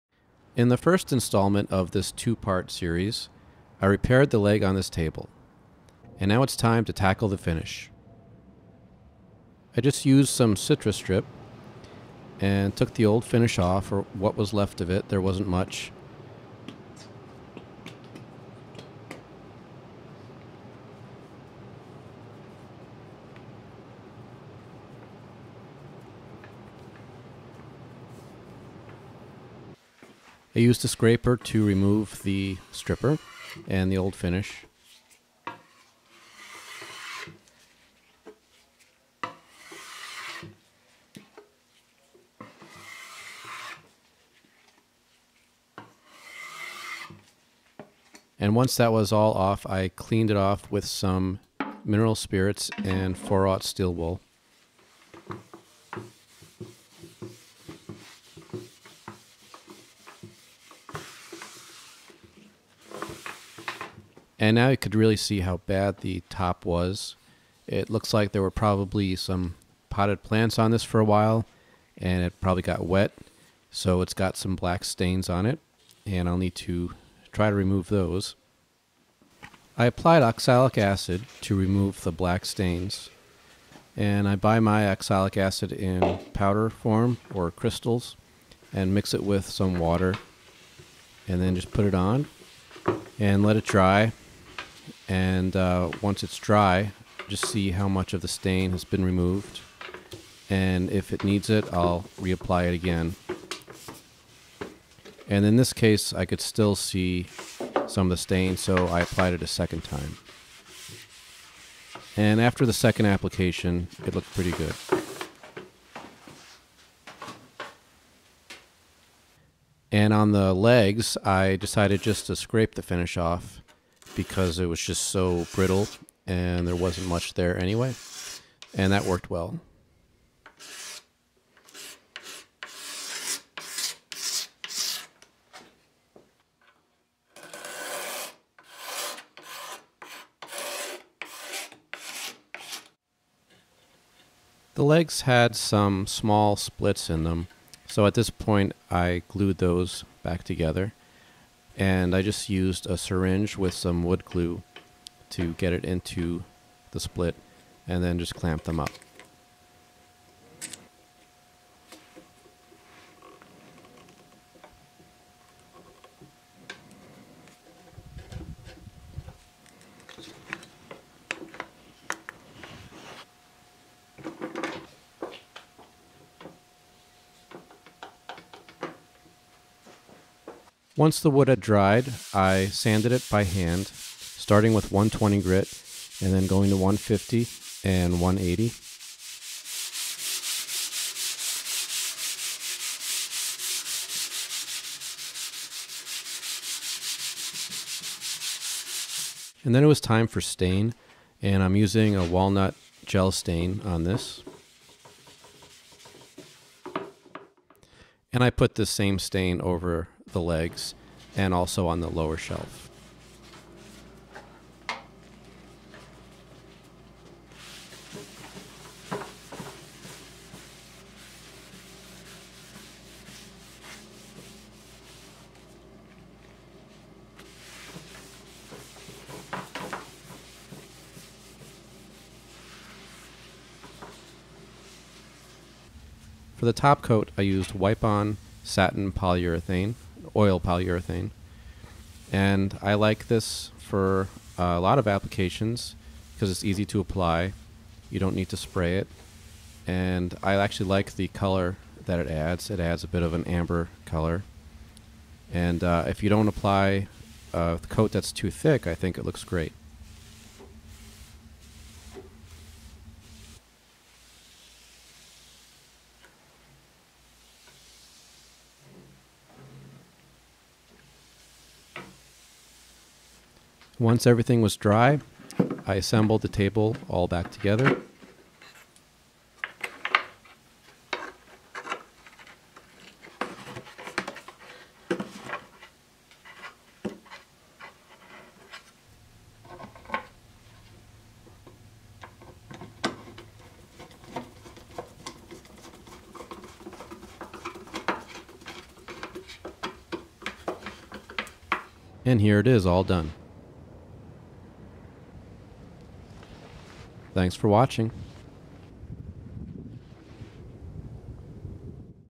Vintage Furniture Restoration | Part 2 - Refinishing an Old Oak Table
In this second installment of this two part series,  I refinish an old oak table.